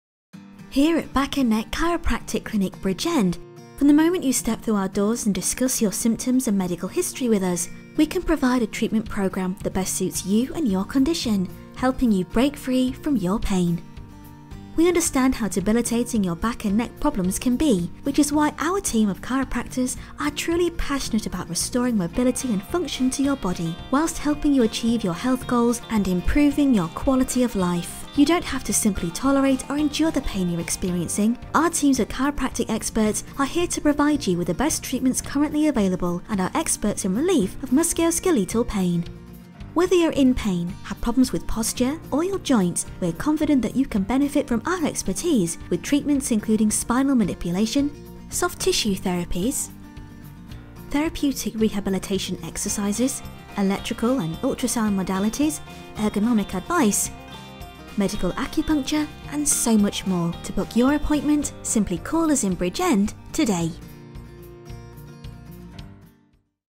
Chiropractor – Back and Neck Chiropractic Clinic Bridgend
Experiencing debilitating back or neck pain? Come and visit Back and Neck Chiropractic Clinic in Bridgend today and speak to our professional team of chiropractors who can help you break free from your pain.